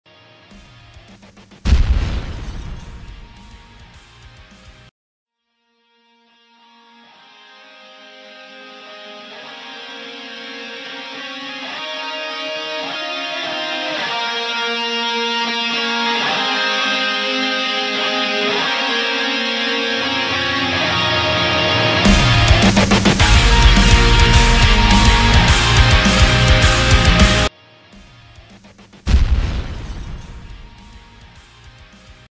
"Blowing Up Car" (VFX) (w/ VFX Breakdown)
Really fucked up on the colors!
This is another little VFX scene I did Learning a shit tone by doing these!